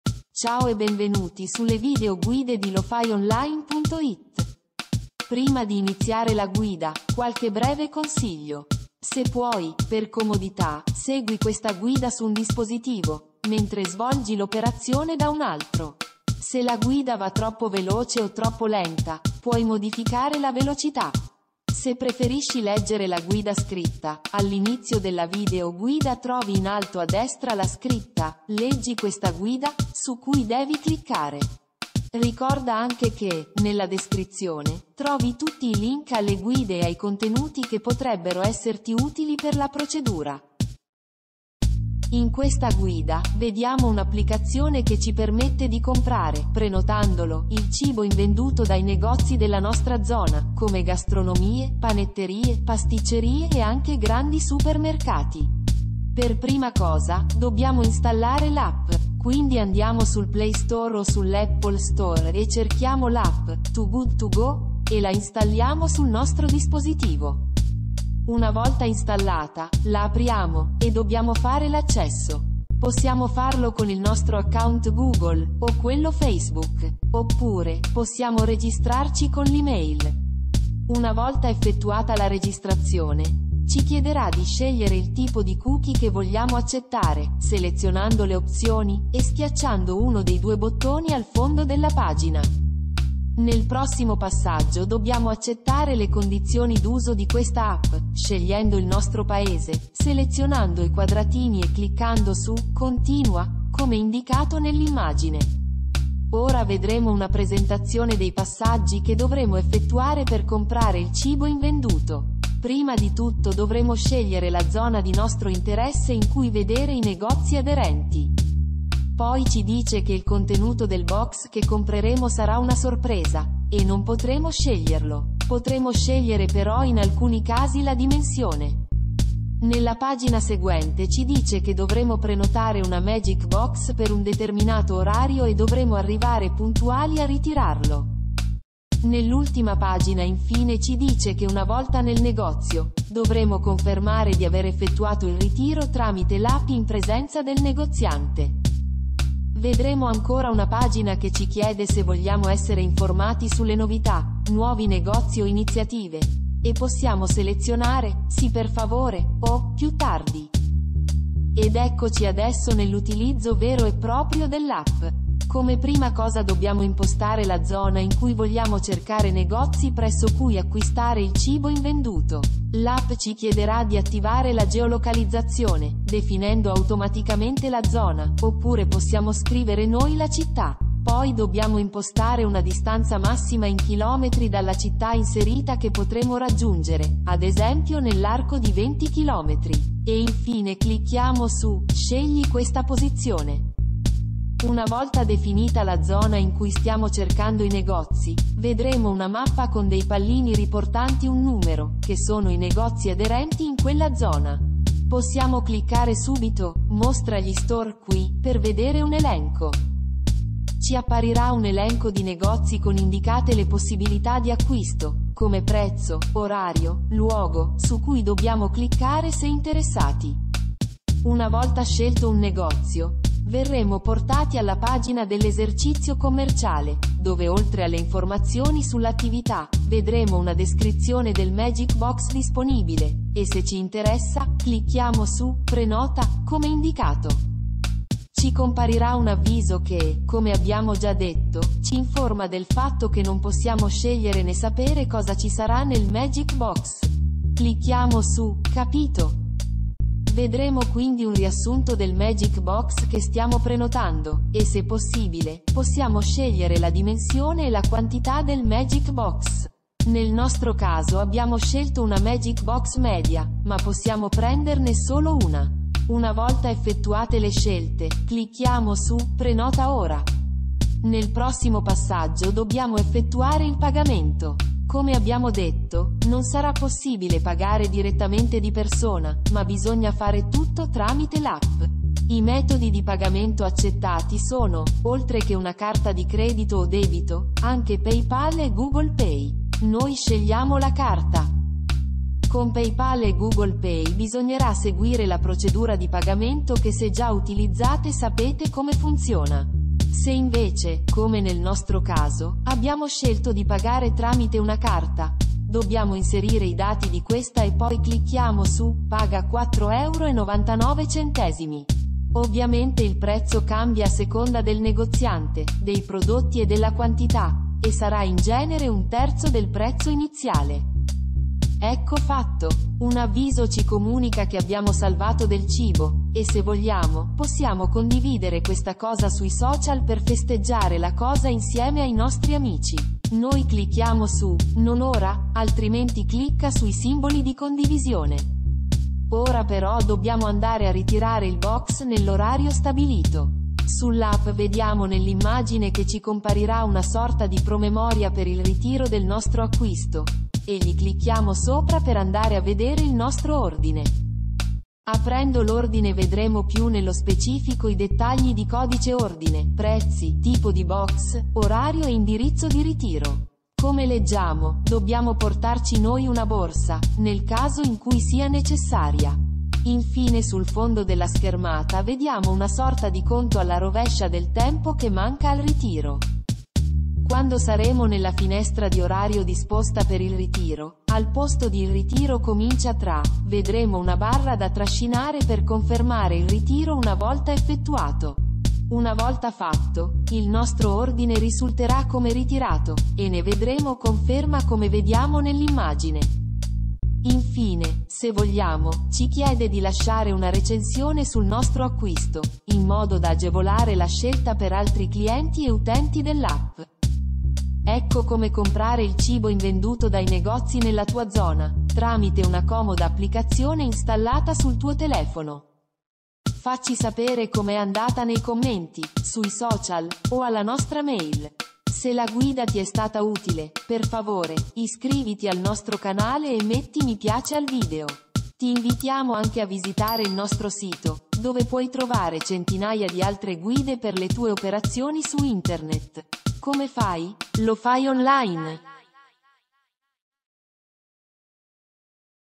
Come comprare il Cibo invenduto dai Negozi con un App
In questa guida vediamo come comprare il cibo ancora buono ma invenduto dai negozi della tua zona tramite una comoda app sul tuo smartphone.

Link a questa guida scritta dove trovi tutti i collegamenti utili